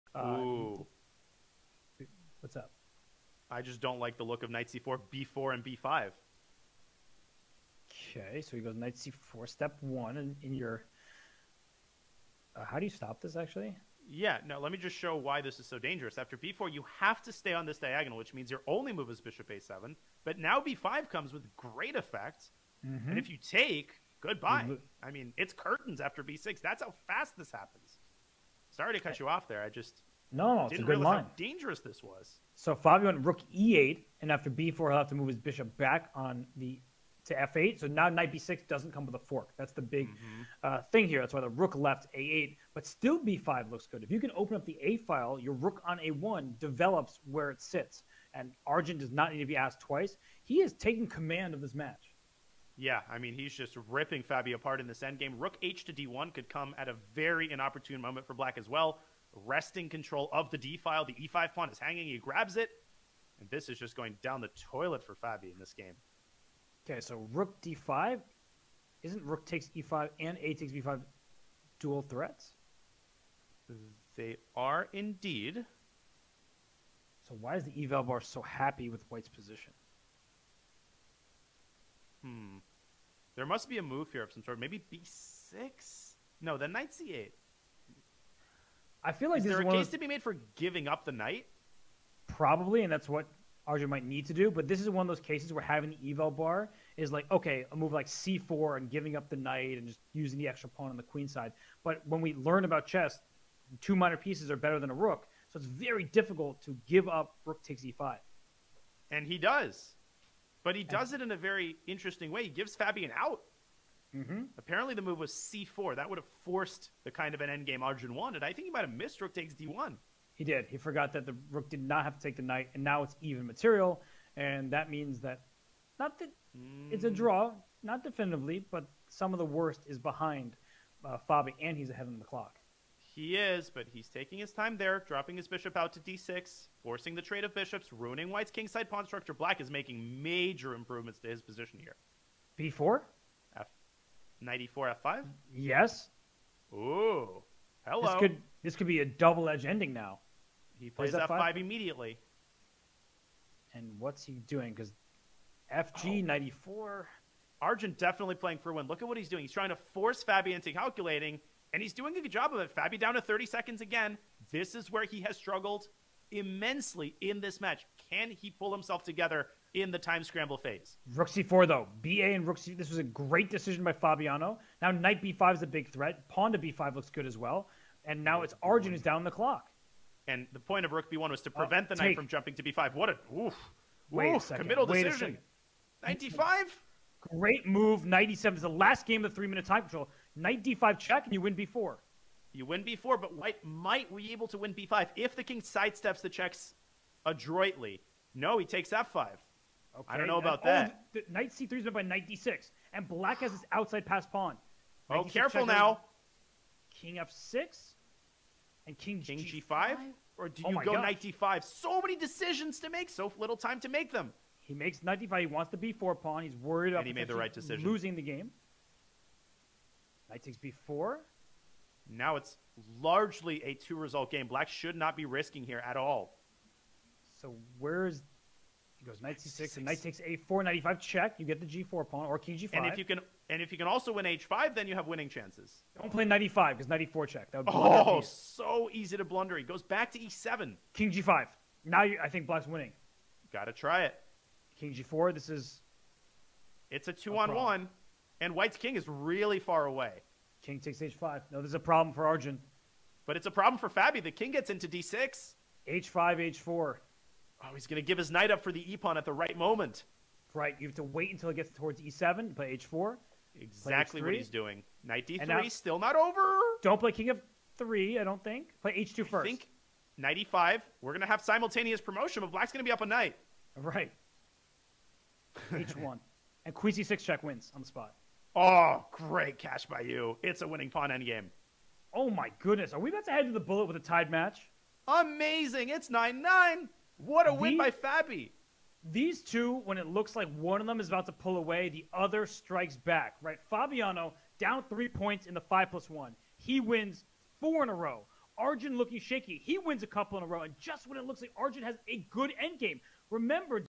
Fabiano Caruana wins HUGE ENDGAME at SCC! ♚
Fabiano Caruana find a way to win a huge game at the 2022 Speed Chess Championship against Arjun Erigaisi!